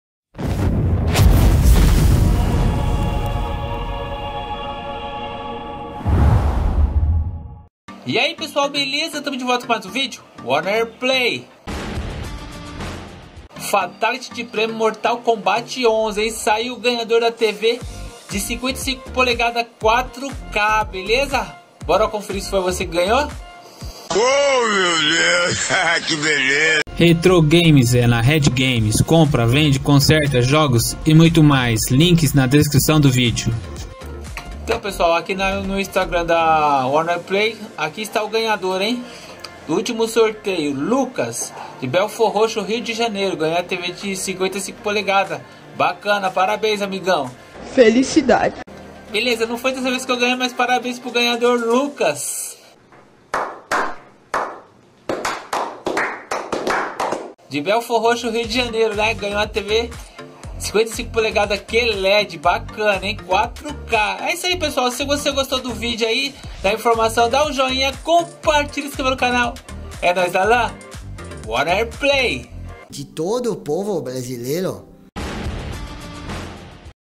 FATALITY DE PRÊMIOS MORTAL KOMBAT 11, SAIU O GANHADOR DA TV Q LED 4K 55 PELEGADAS WARNER PLAY
►FONTES: FATALITY DE PREMIOS MORTAL KOMBAT 11 WARNER PLAY
Muito obrigado.

Eu estou usando o Ganhe na Tela para ganhar créditos para celular pré-pago! Use